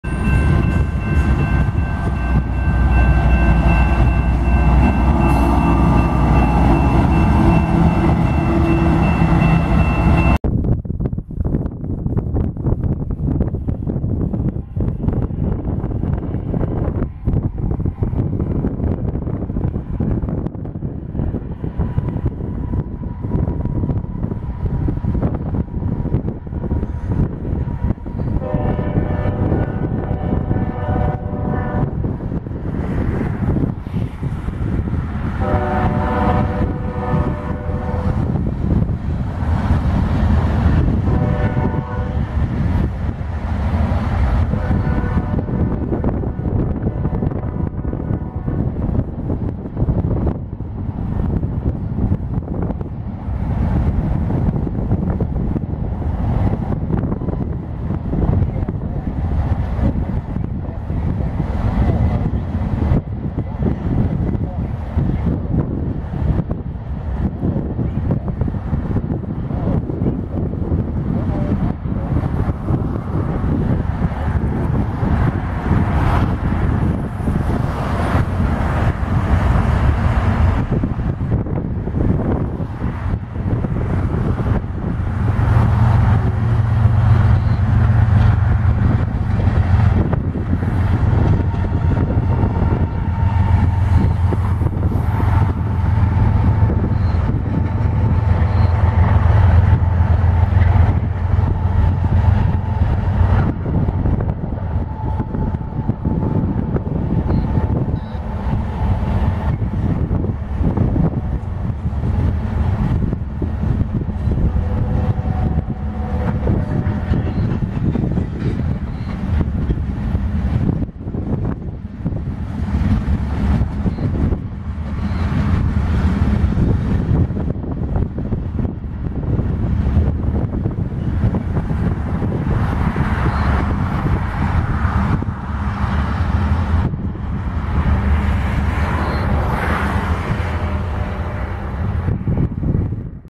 CSX 850 Canoe
Northbound Freight In Canoe, AL in November 2022